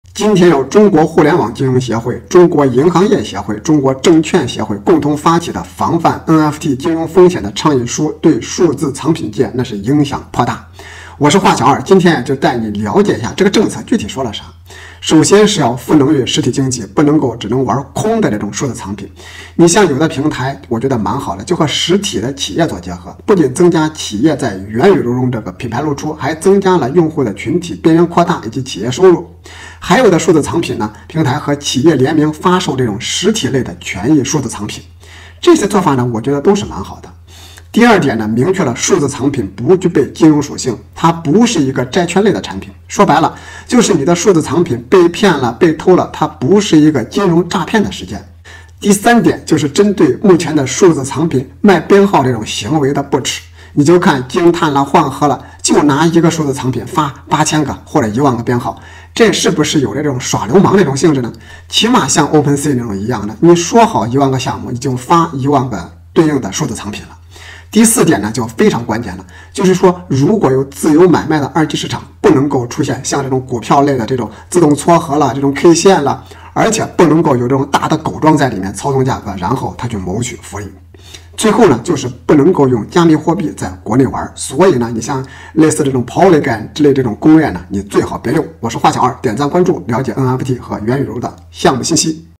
解读互联网协会、银行协会、证券协会防范NFT金融风险
今天中国互联网金融协会、中国银行业协会、中国证券协会共同发起的防范NFT金融风险倡议书对数字藏品界影像颇大。我是画小二，今天就带你解读一下政策说的啥。